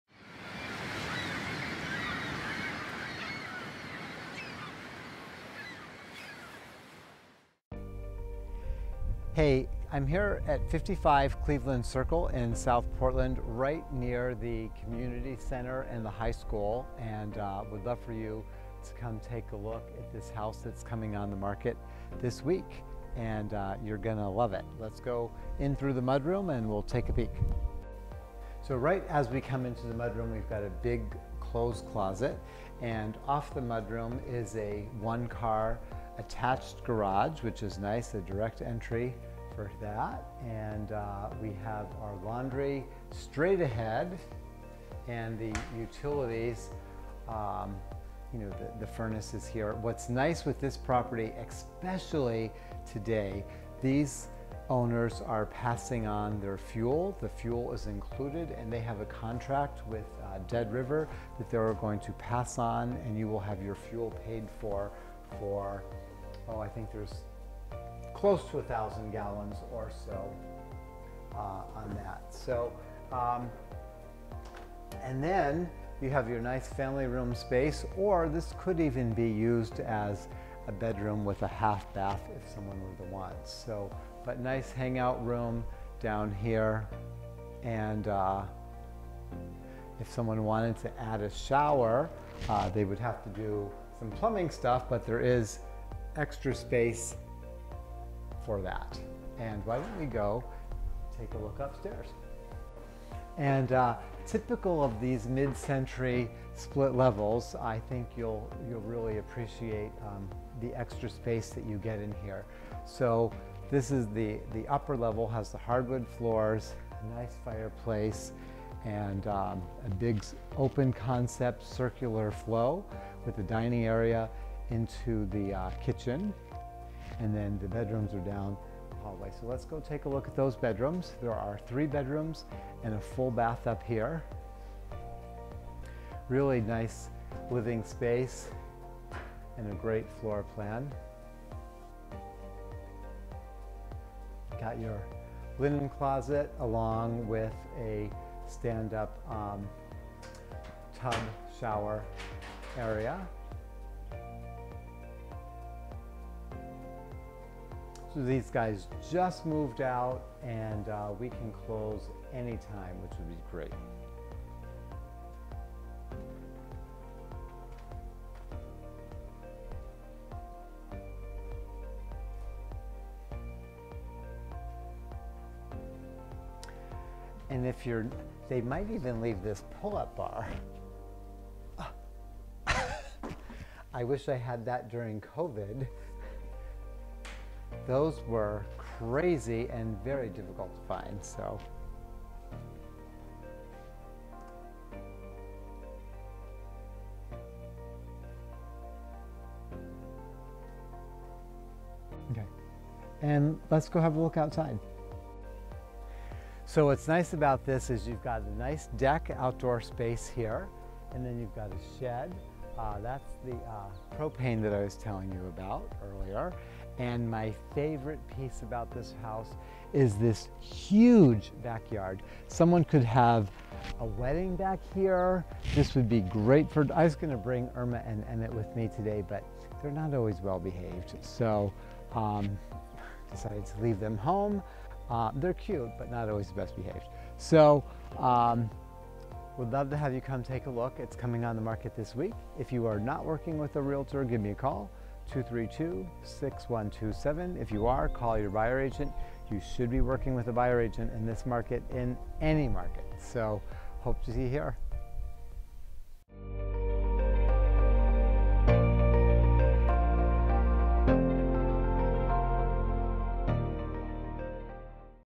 55 Cleveland, South Portland, Maine
SOLD - $430,000
3BR/1.5BA home in great South Portland neighborhood.